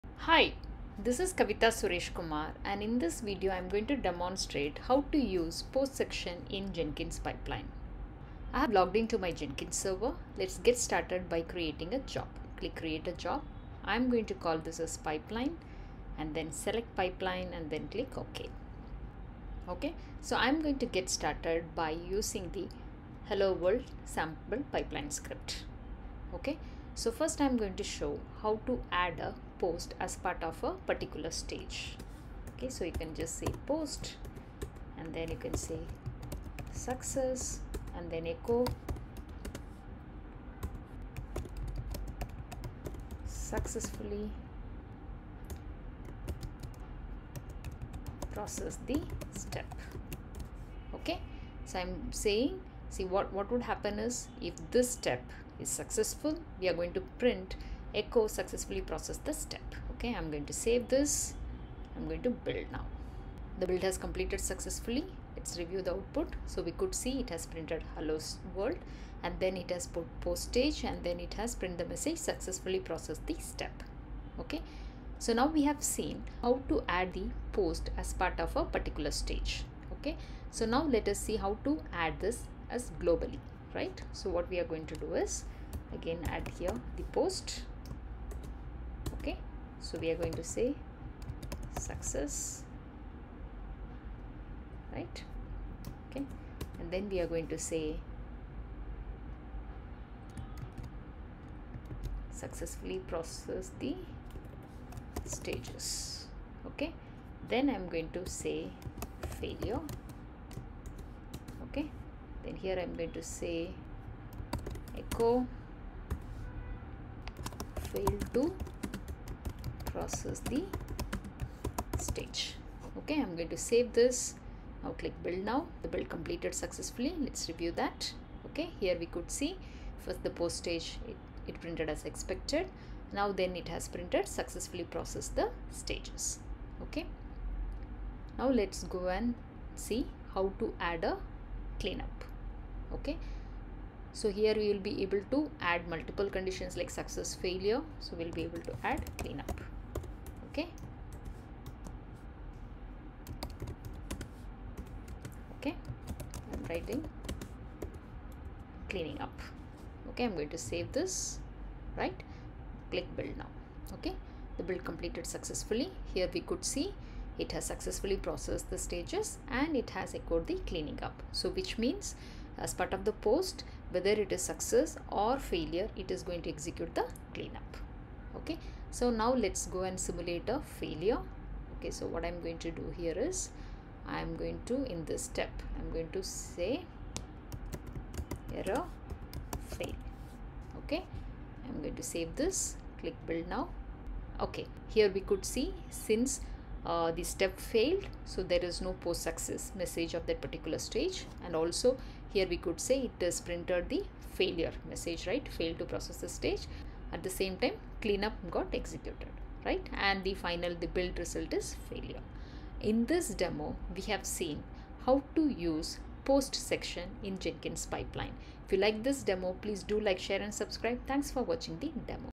Using Post Section in Jenkins Pipeline
In this video, you will learn how to use post section in Jenkins Pipeline